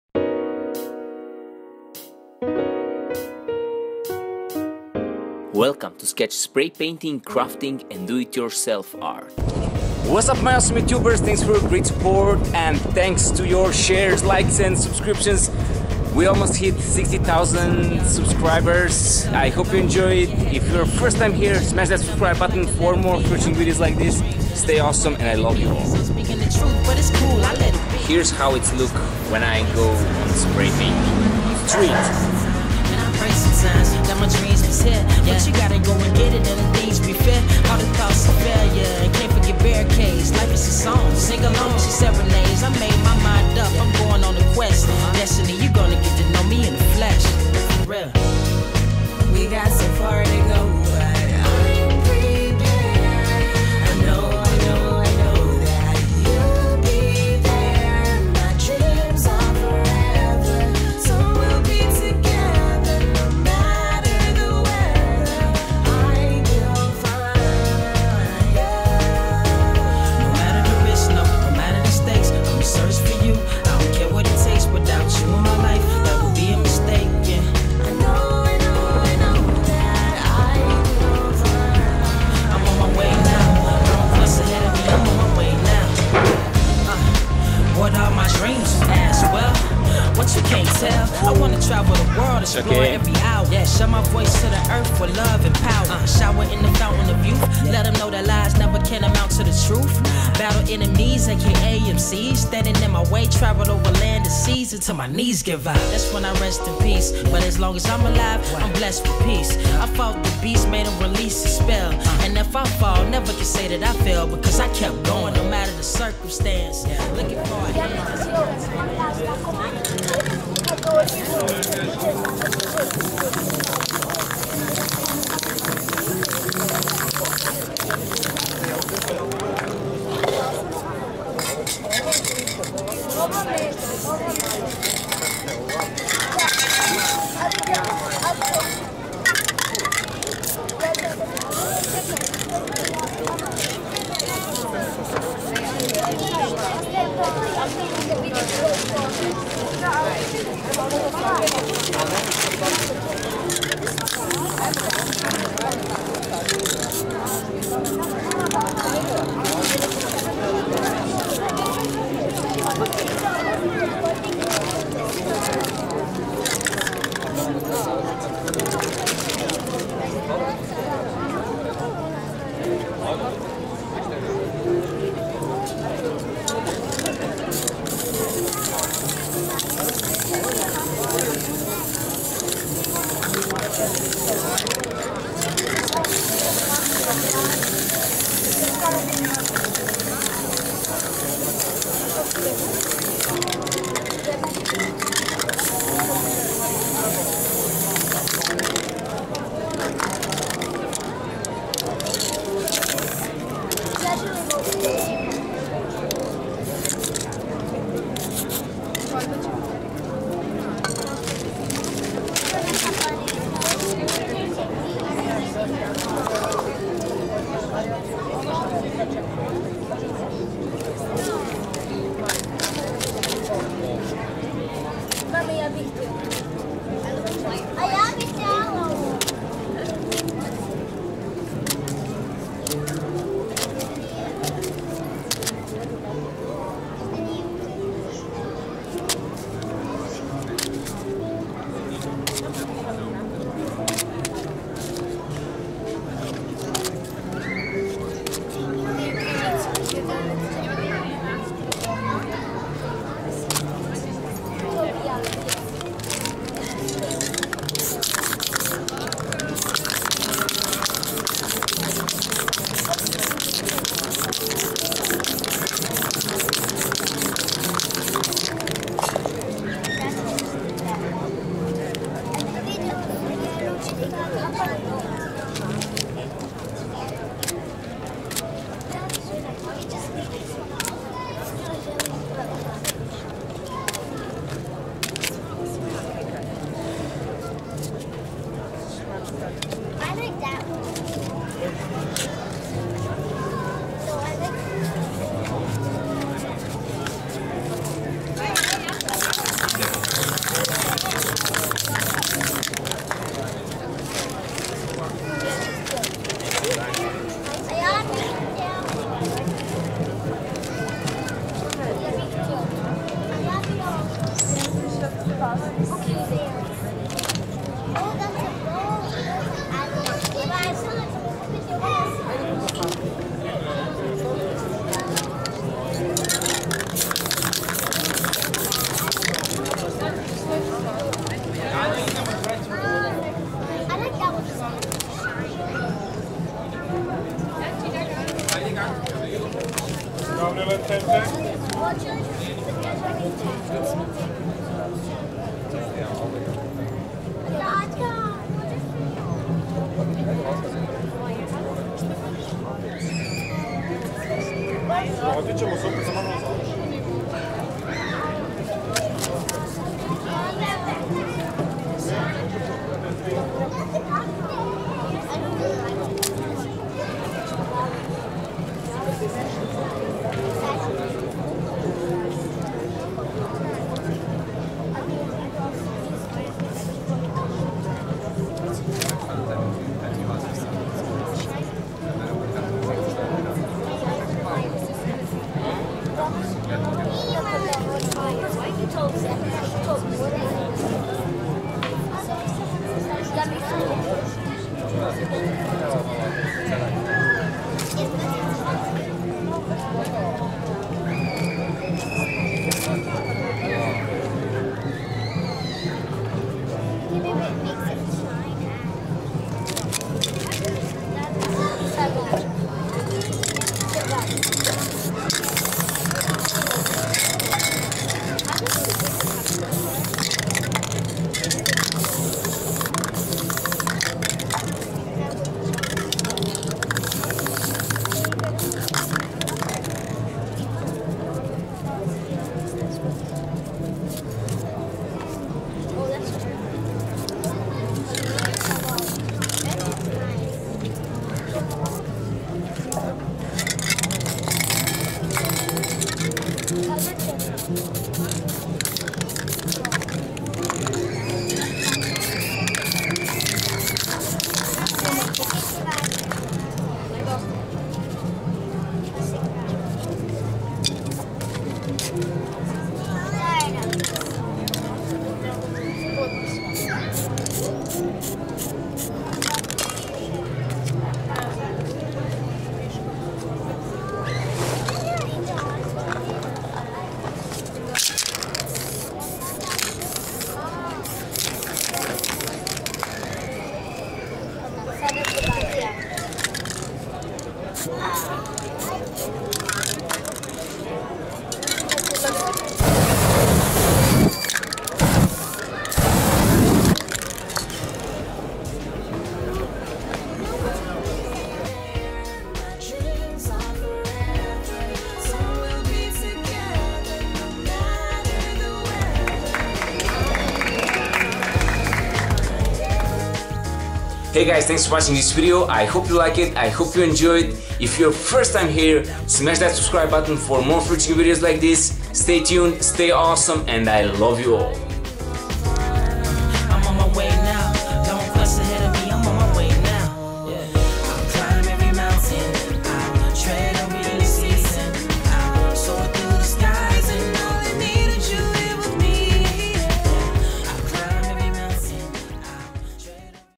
SPRAY PAINT ART by Skech - Spray painting on the street
-Firs half of this video is me trying to make vlog :D and 2nd part is tutorial how to make step by step from scratch SPRAY PAINT ART that u probably already saw but instead of water ship was on clouds :)
*I record this video with Sony action cam and here is link where u can faind it:

Spray paint ART by Stefan Acanski Skech